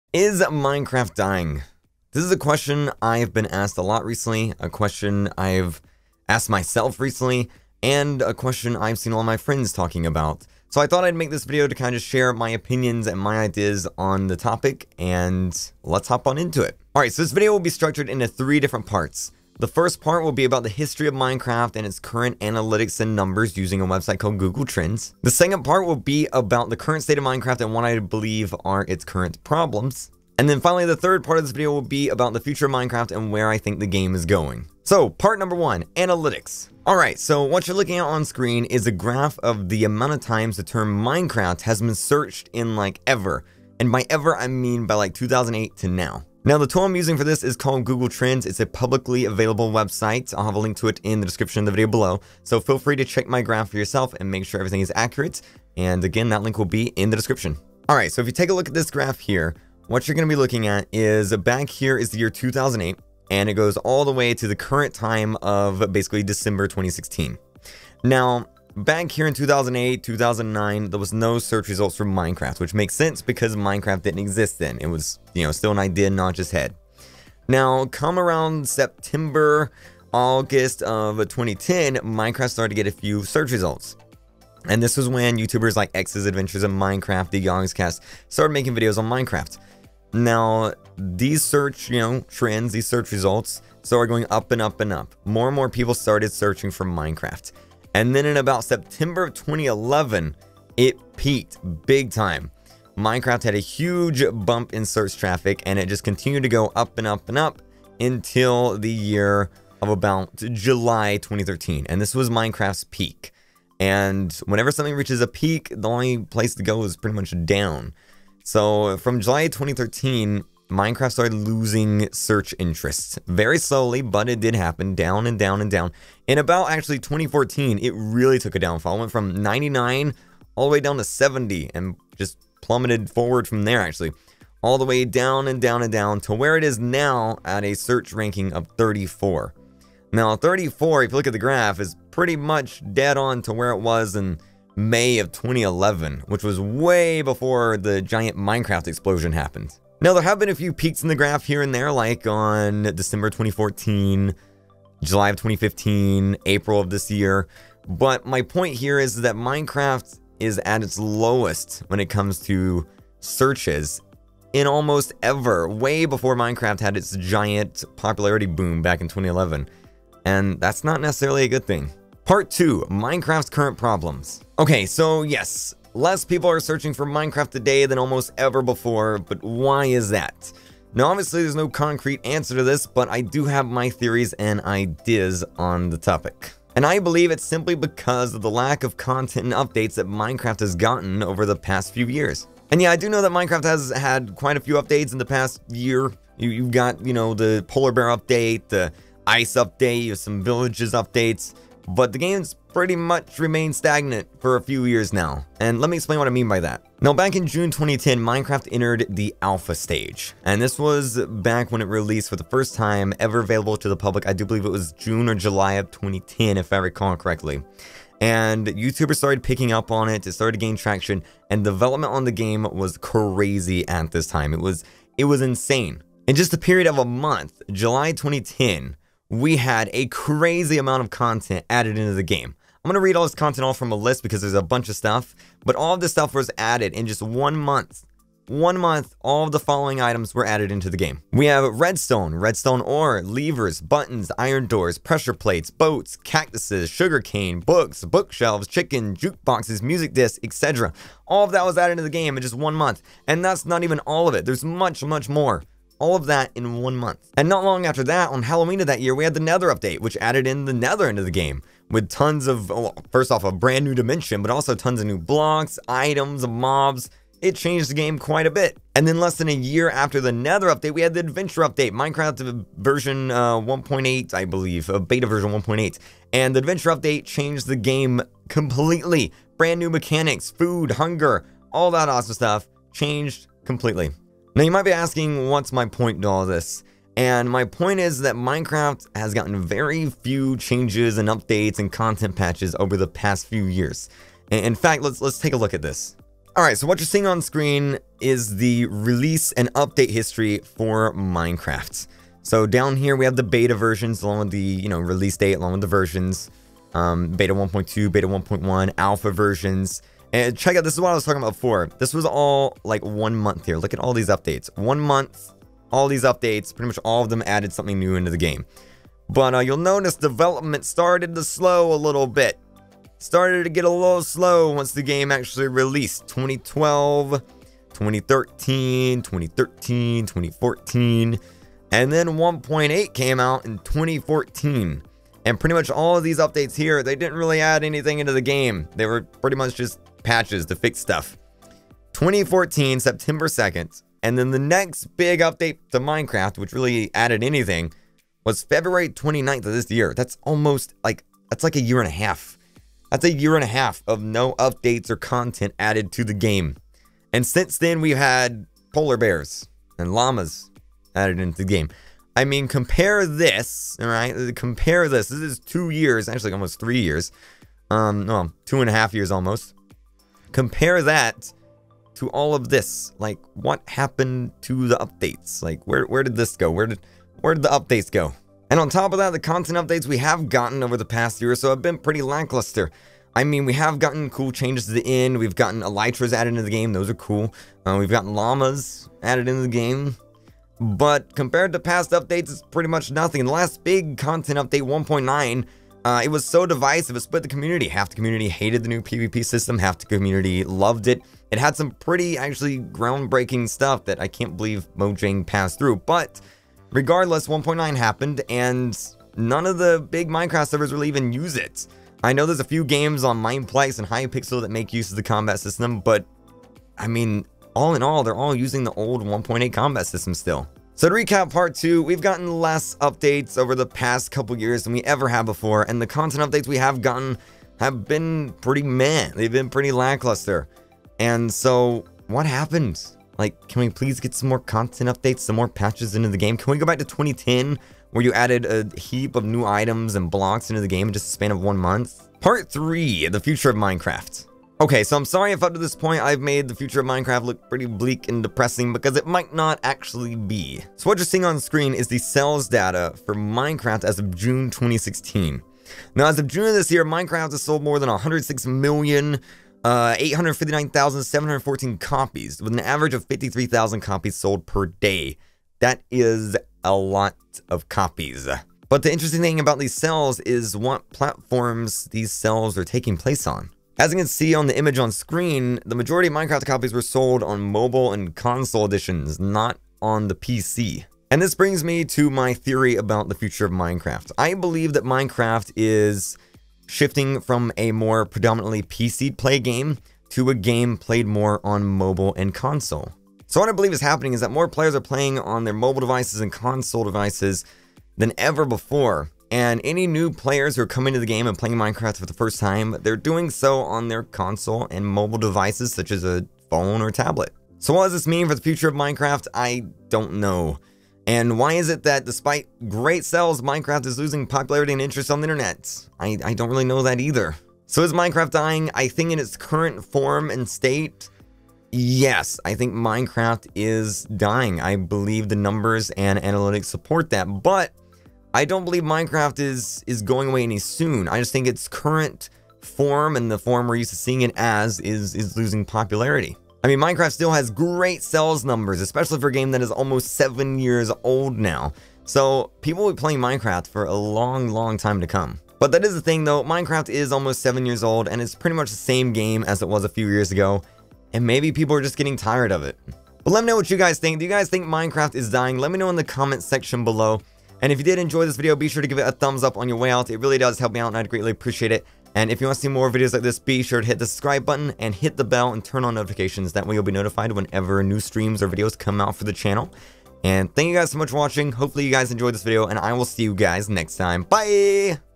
IS MINECRAFT DYING!? Let's Talk About Minecraft's Popularity, Problems, and Future
We LIVESTREAM every day at 10 AM EST and 9:30 PM EST (except Monday nights)! Join us on YouTube Gaming!

💙 SOCIAL MEDIA! 💙